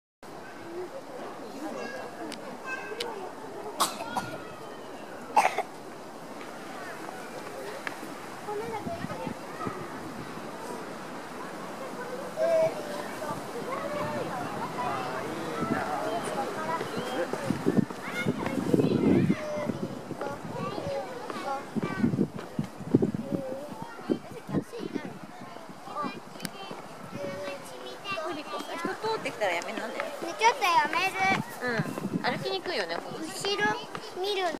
一歩一歩
金沢動物園のシロイワヤギのペンケ。
蹄を治療中であり、17歳の高齢ということもあり、
ペンケは一歩ずつゆっくりと歩きます。